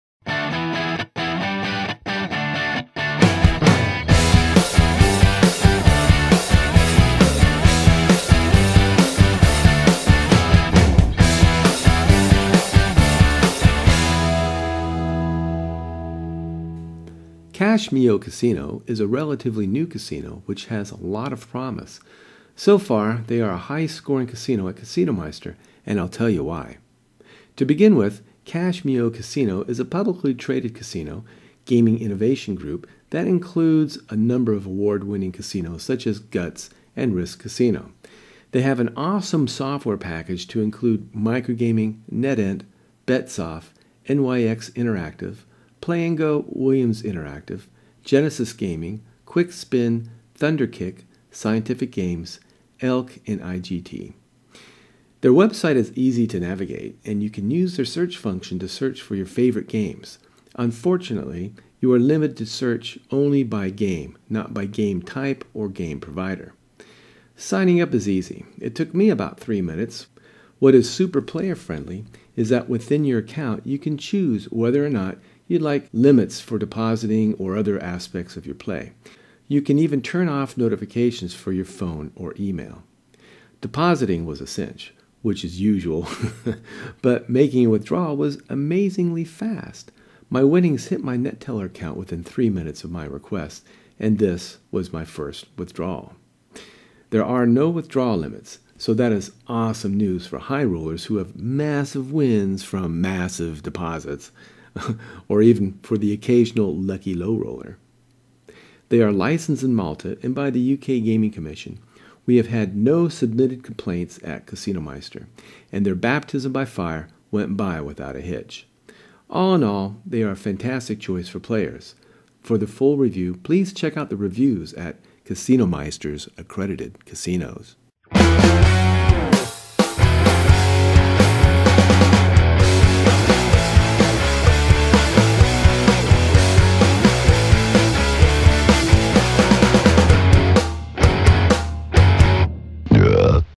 Cashmio Casino Review by Casinomeister
Casinomeister's review on Cashmio casino - payout times, withdrawal limits, all this and more!